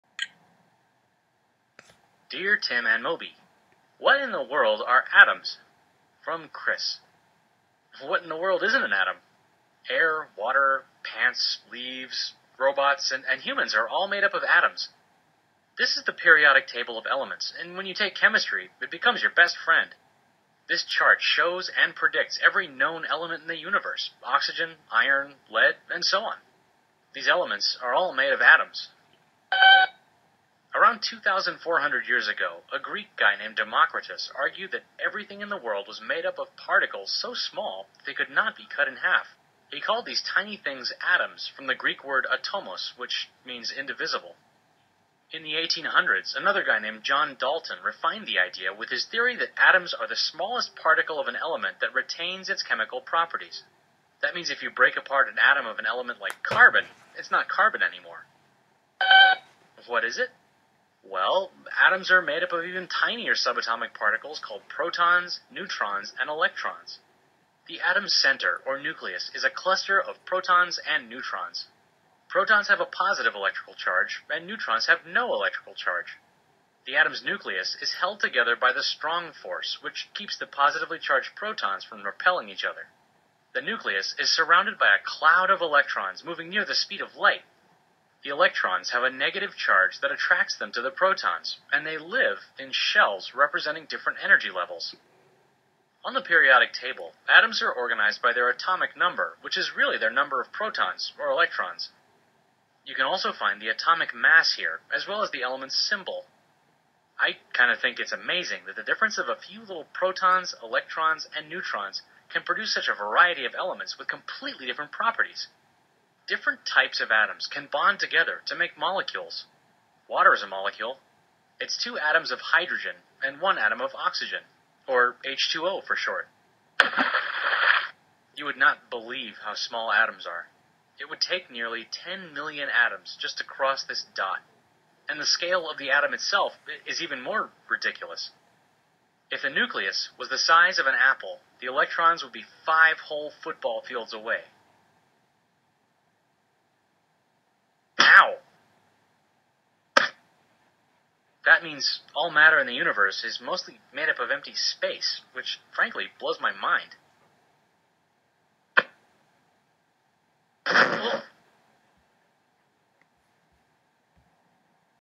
Atoms
Video talking about the atomic structure.